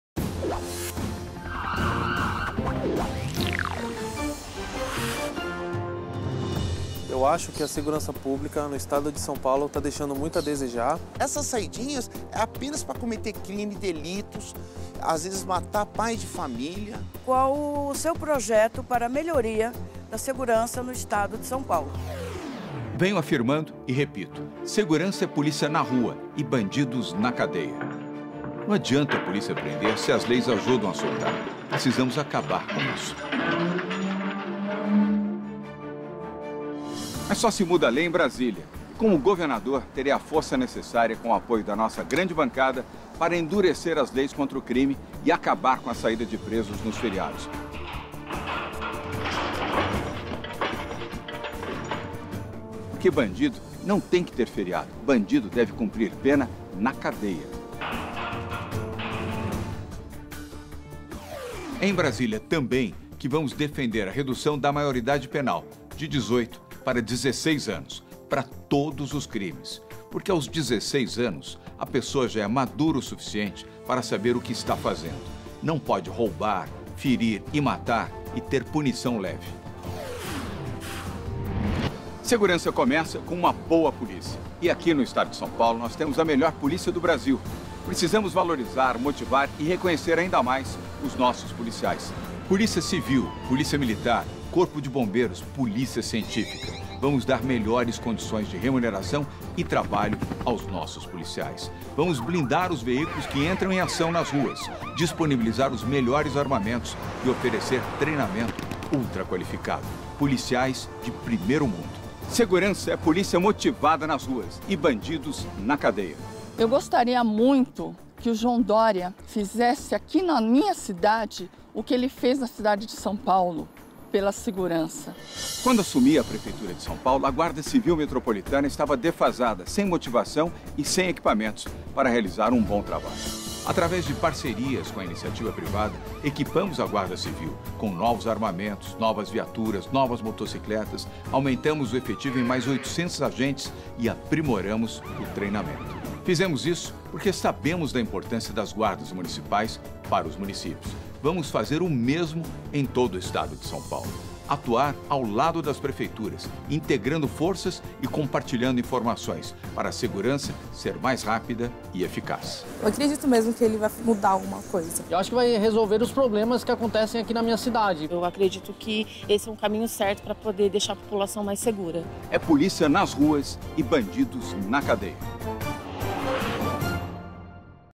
Programa 11 - João Doria
Coligação Acelera SP: PSDB, PSD, PRB, DEM, PP, PTC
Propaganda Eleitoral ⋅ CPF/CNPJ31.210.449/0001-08
Eleição 2018 João Agripino da Costa Doria Junior Governador

Afirmo e repito: segurança é polícia na rua e bandidos na cadeia. Conheça nossas propostas para a segurança pública do Estado de São Paulo.

Você conhece a minha história?
Quem conhece, aprova!
Fake News? Conheça a Verdade!